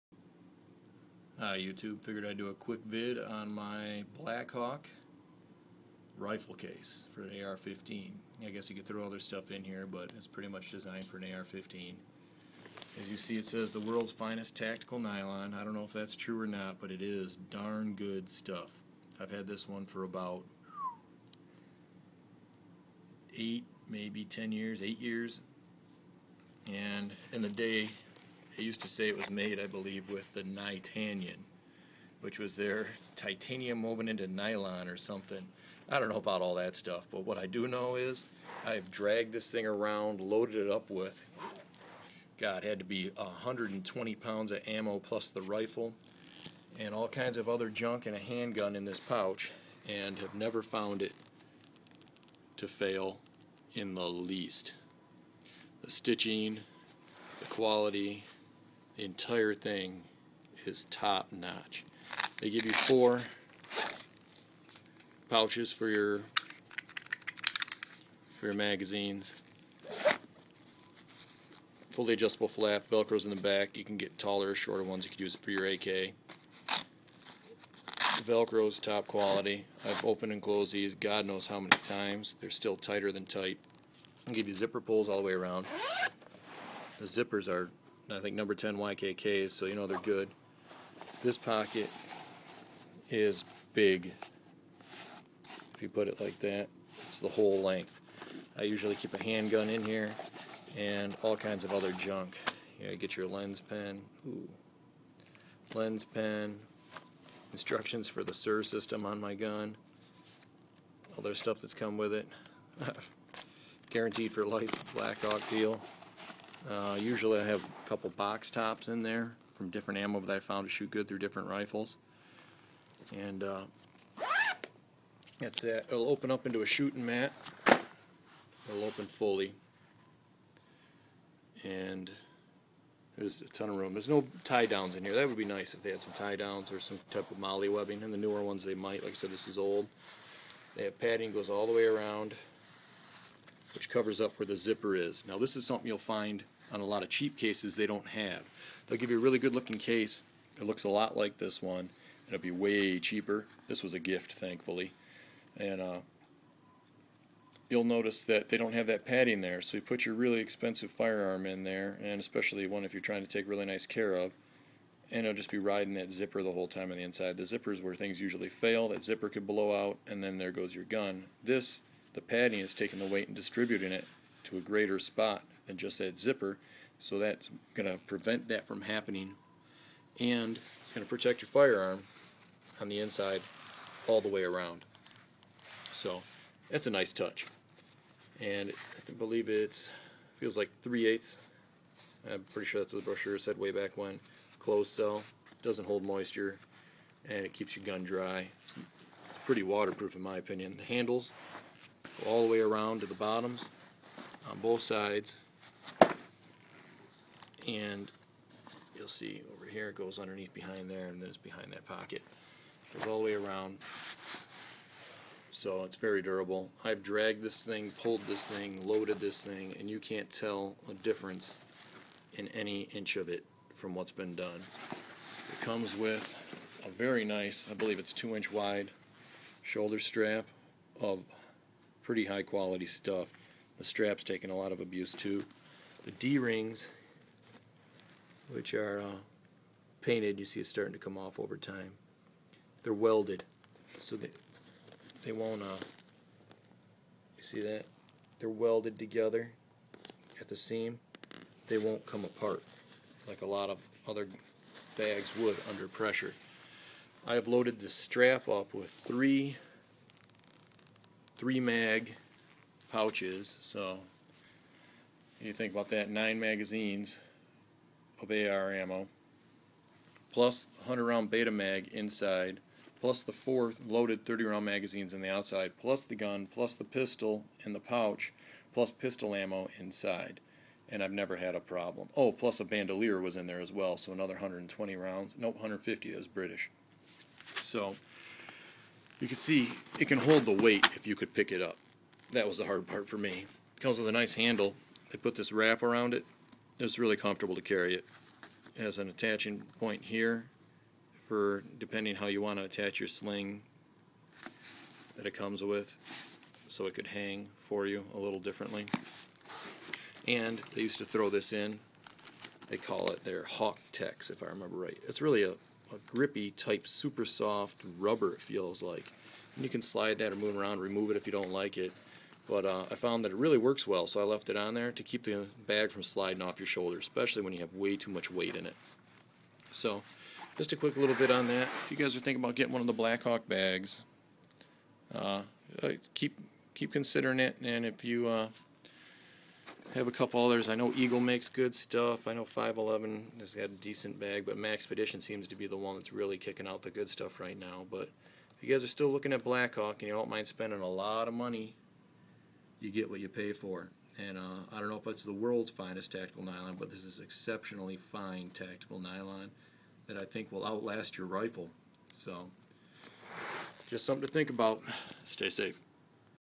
Blackhawk Tactical Rifle Case
A few thoughts about this awesome bag. I've had it for about 9 years and it's seen a ton of use, after all of this it still looks more or less brand new.